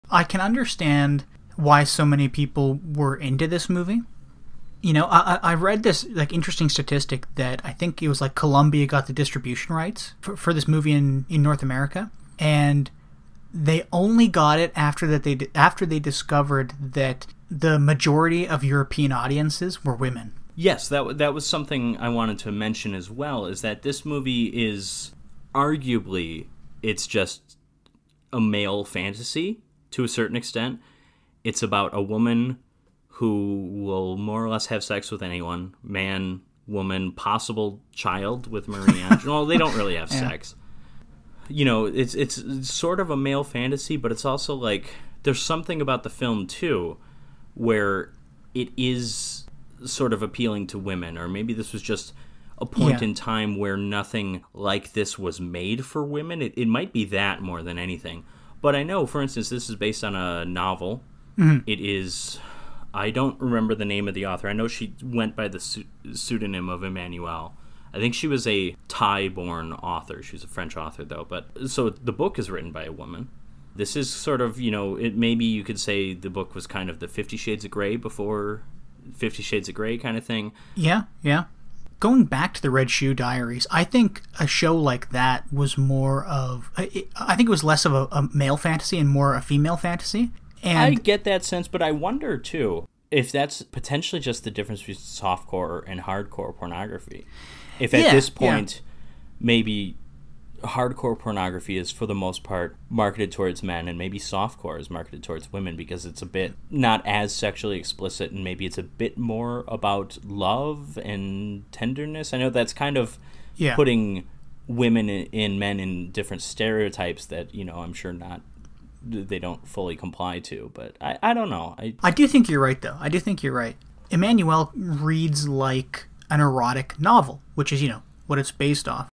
Emmanuelle (1974) - For the Ladies
A discussion of the massive success of Just Jaeckin's Emmanuelle, featuring a brief discussion of the history of pornography, from our Emanuelle/Superdragon vs. Superman episode